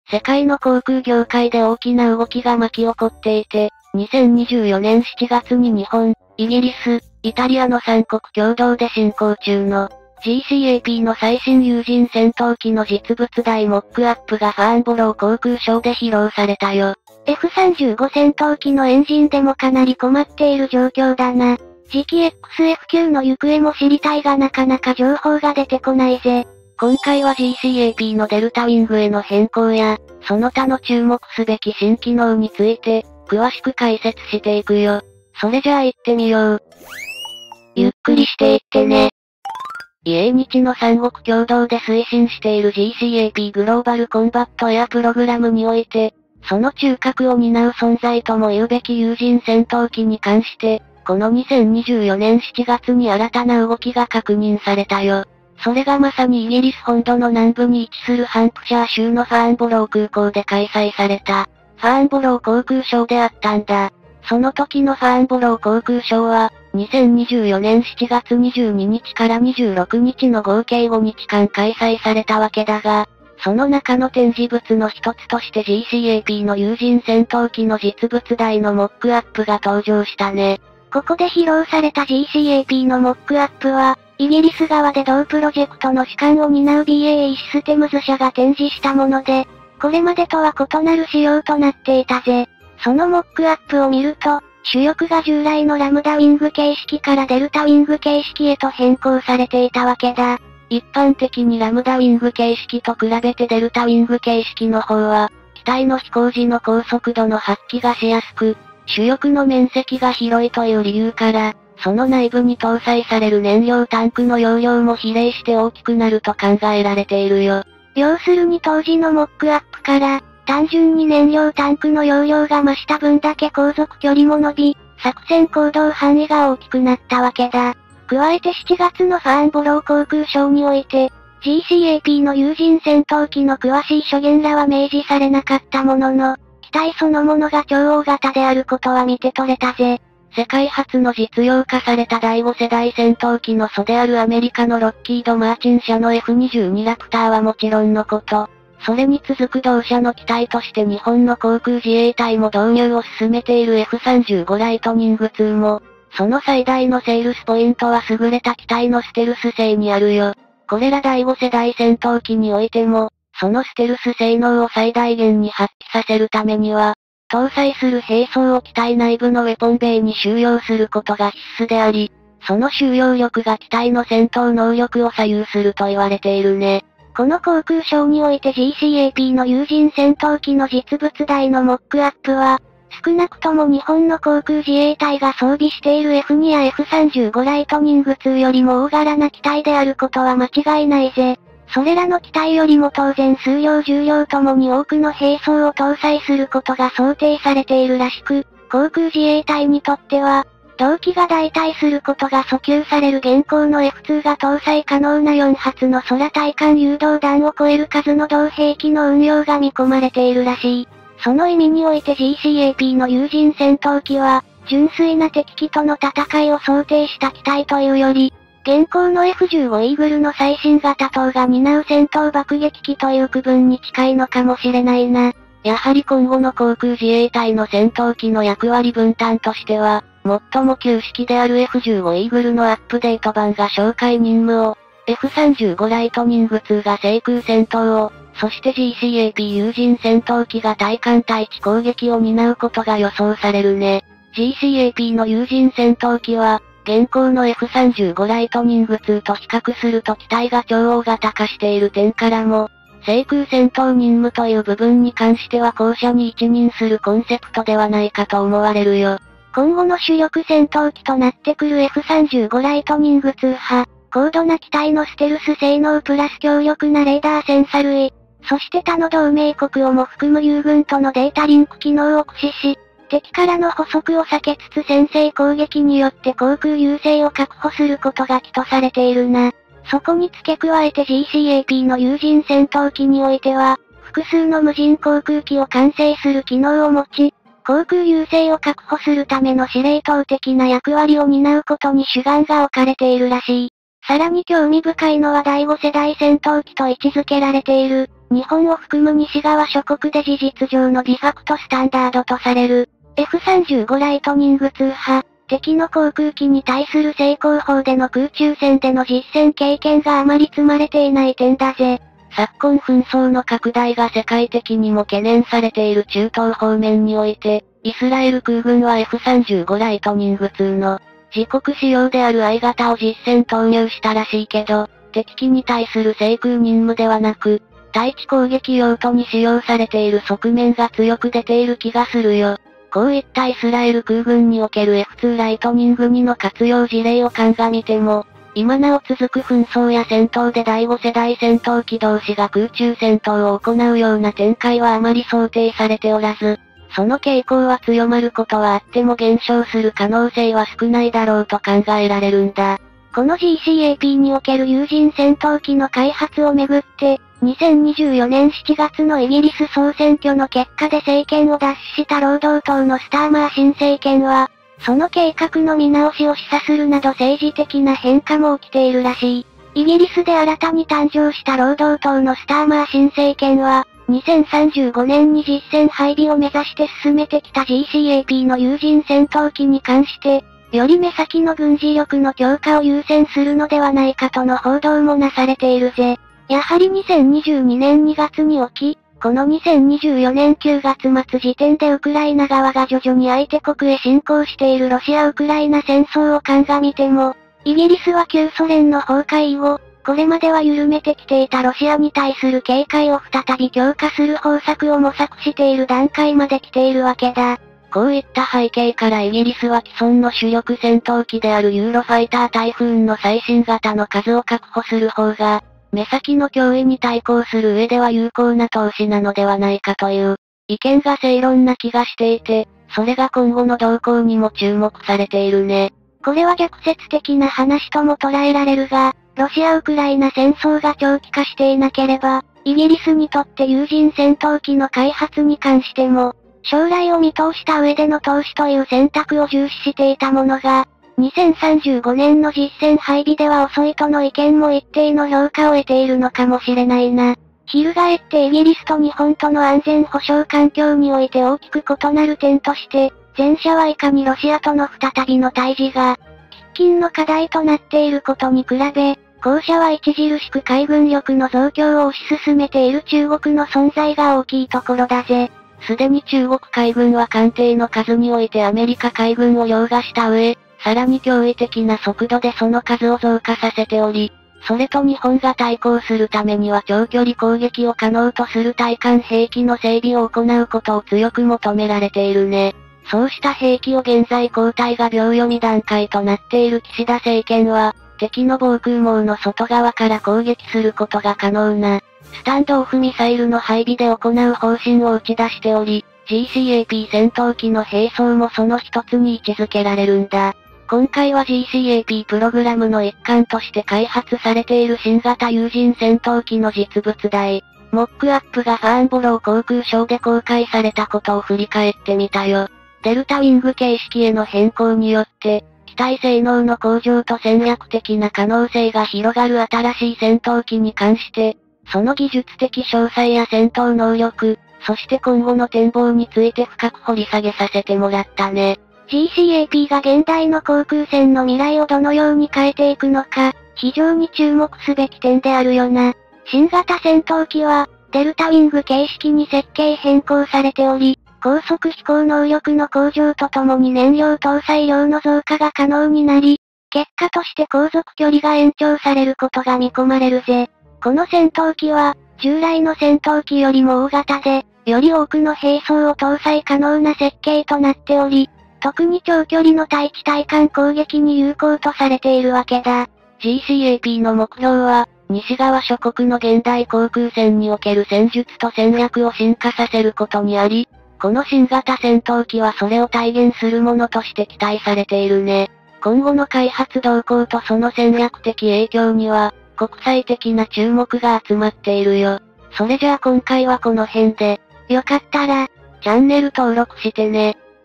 【ゆっくり解説・軍事News】自衛隊ついにF3戦闘機GCAPモックアップか！翼が超異変デルタウイングに国連ビビる次期XF9驚異的開発スピードが悪魔的？
引用・転載「出典：海上自衛隊ホームページ」
引用・転載「出典：航空自衛隊ホームページ」
引用・転載「出典：陸上自衛隊ホームページ」